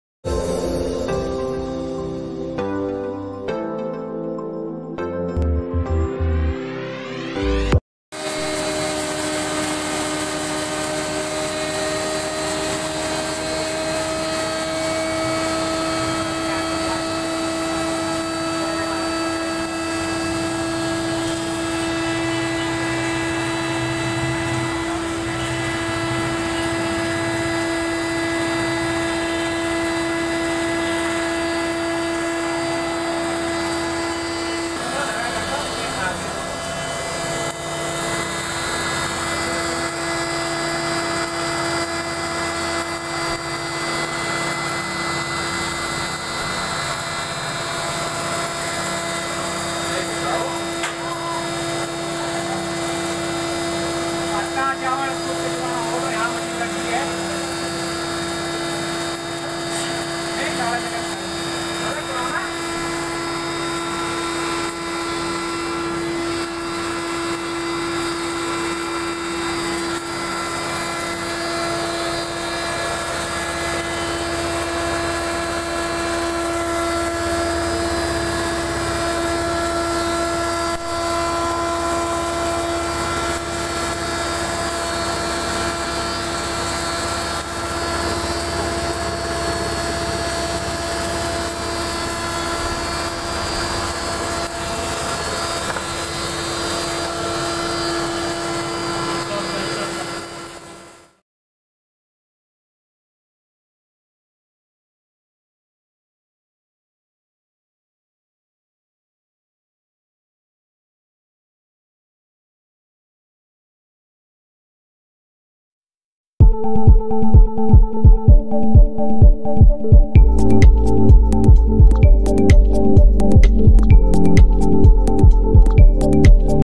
Rice Cum Flour Mill Machine | Combined Rice Mill
* SPECIFICATIONS :
▪︎ RICE cum FLOUR Mill (2 in 1)
▪︎ Motor : 3HP Single Phase
▪︎ MODEL : RI-RF 86
▪︎ Production (Rice Mill) :  180 Kg/h
▪︎ Production (Flour Mill) :  250 Kg/h
▪︎ Spindle Speed (Rice Mill) : 1600 rpm
▪︎ Spindle Speed (Flour Mill) : 4800 rpm
▪︎ Application : Rice, Corn, Wheat, Coffee,
                             Millet

Lakshya Agriculture Equipment Bhopal -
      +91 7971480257